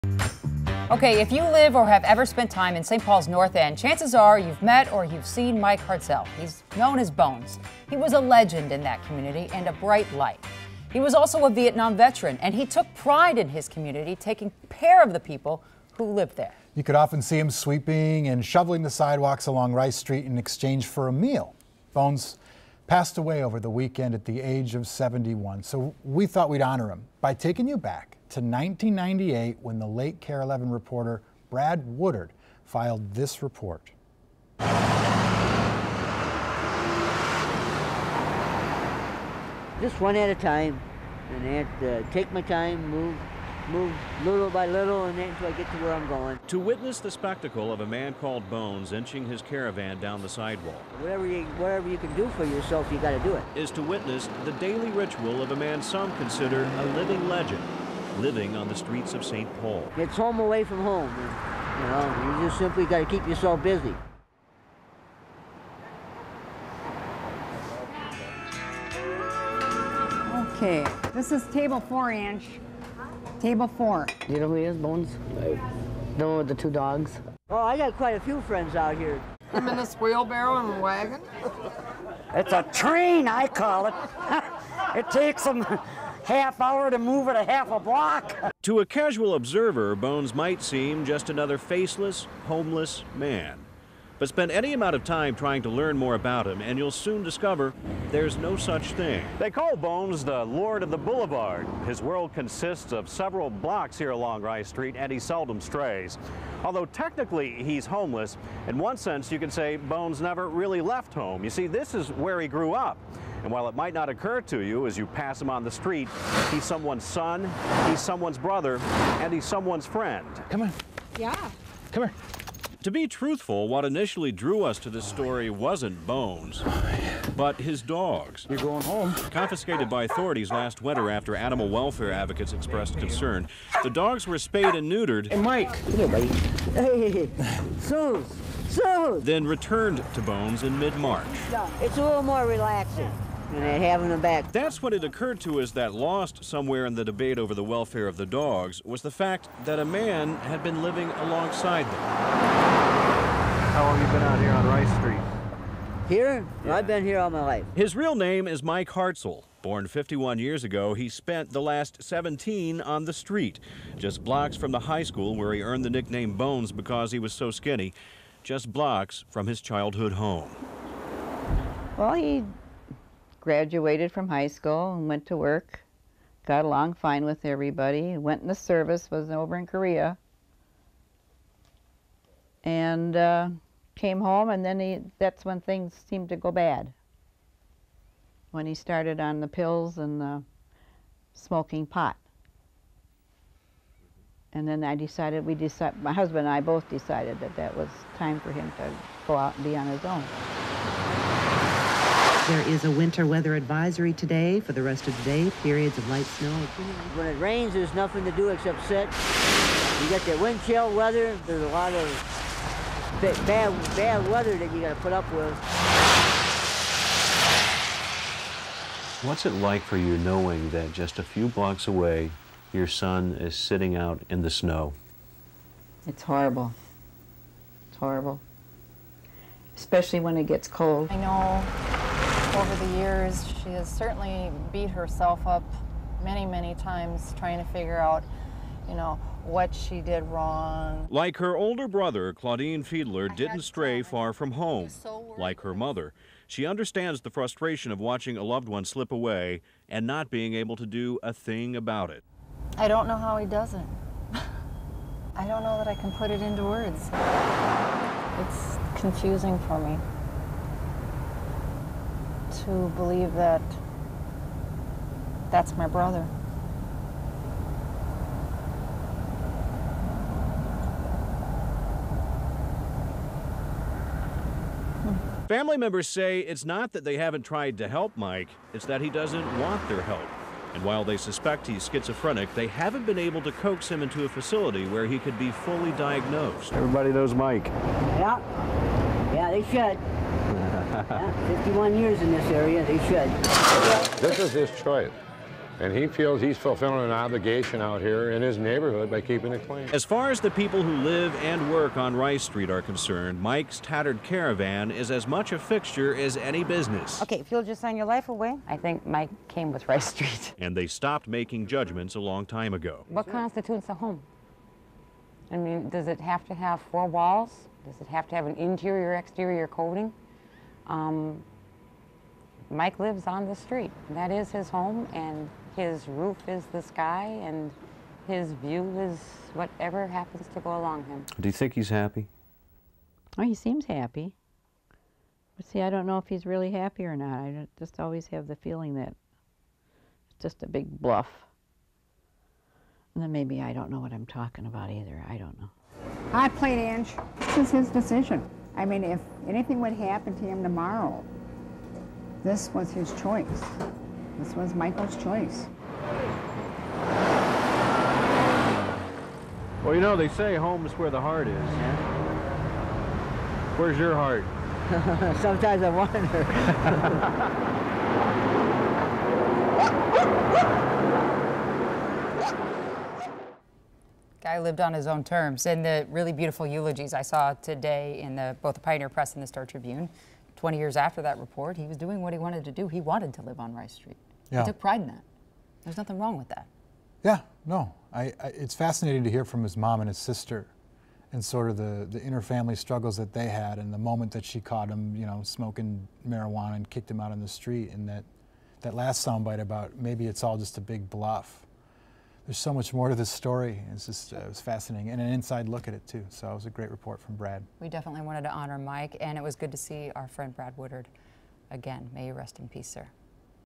Rice Street fixture Mike 'Bones' Hartzell passes at 71
Mike "Bones" Hartzel, a man as identified with Rice Street as any of the bars, cafes or small businesses that line its sidewalks, died Sunday at 71, just a few miles from the street he called home.